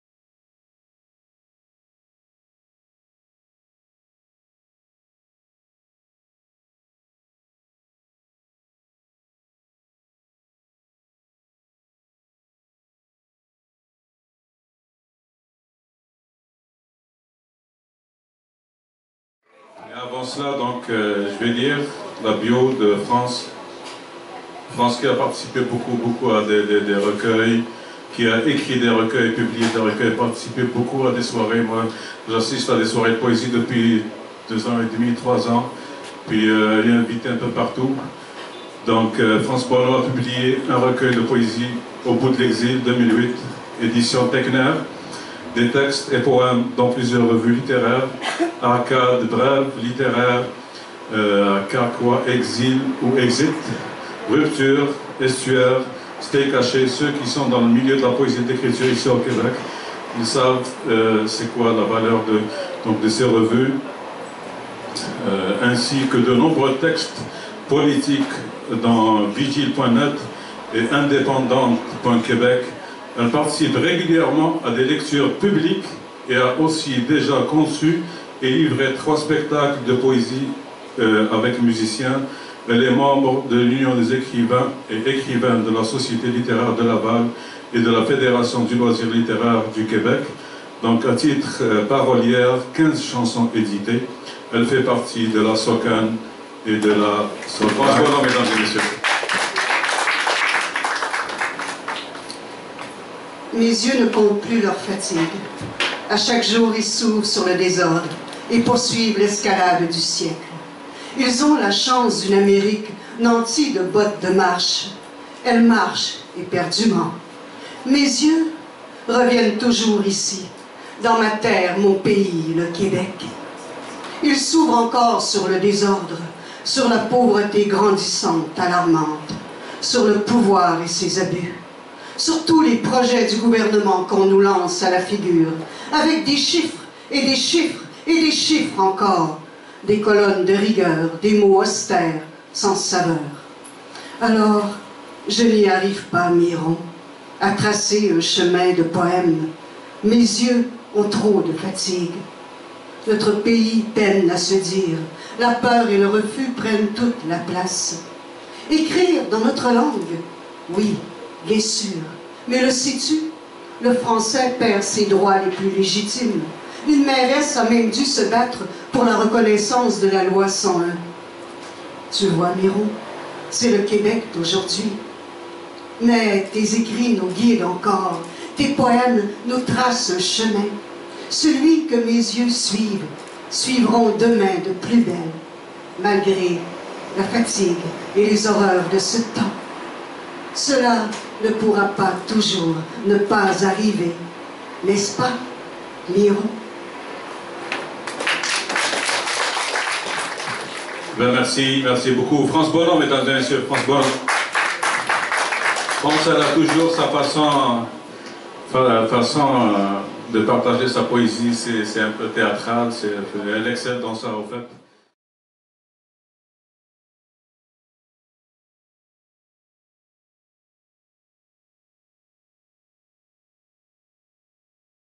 France Bonneau au lancement du collectif "Hommage aux femmes poètes" - Le vendredi 20 mai 2016
Organisé par : Akim & Maryse Cournoyer
Animation : Akim